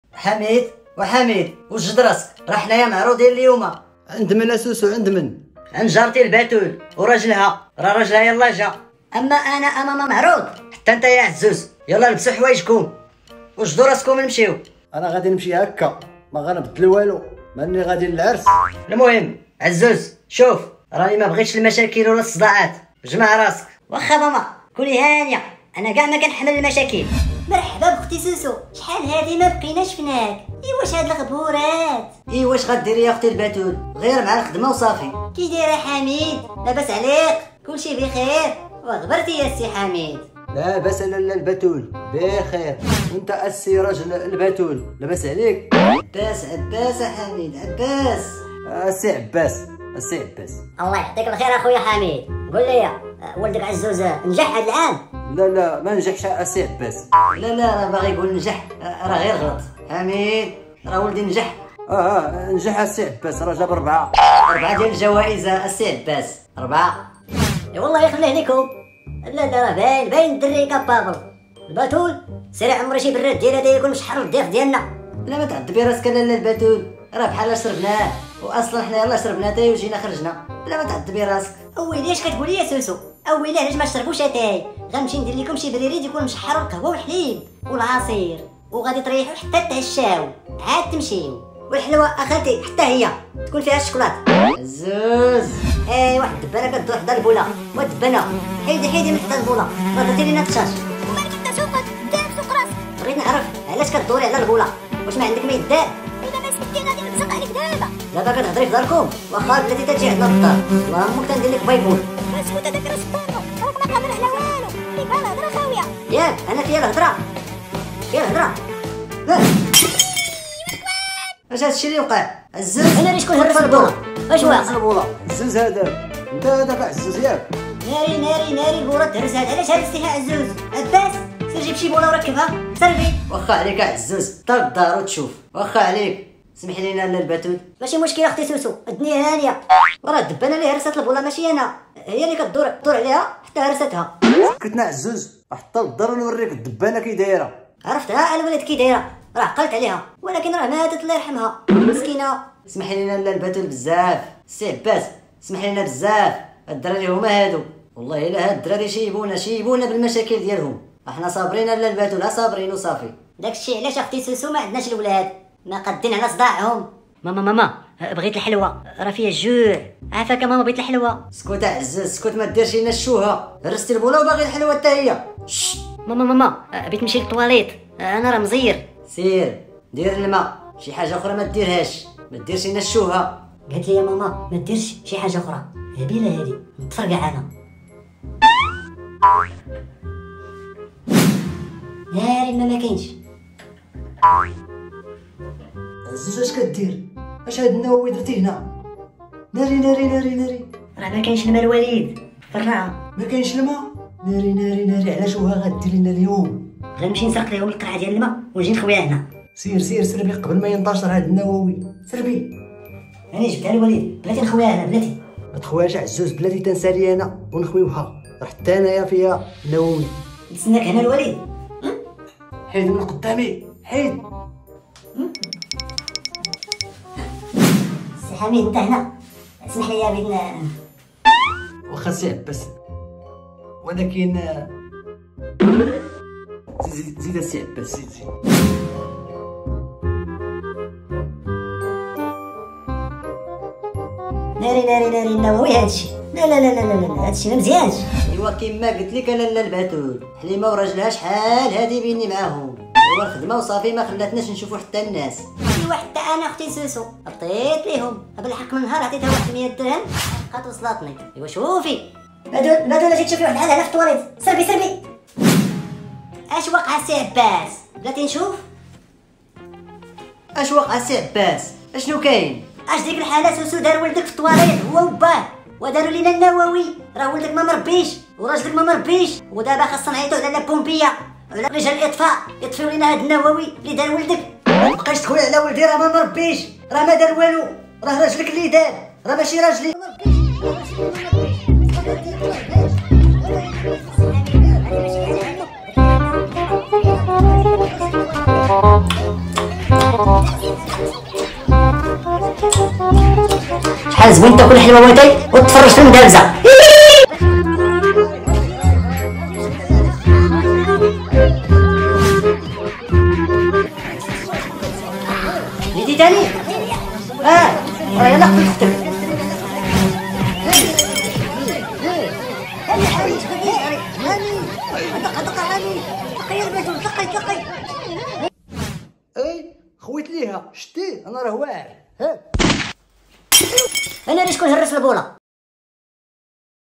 فاش ضاربنا مع الجيران
الجيران عرضو علينا للعشاء ولاكن قامت الكارثة وضاربنا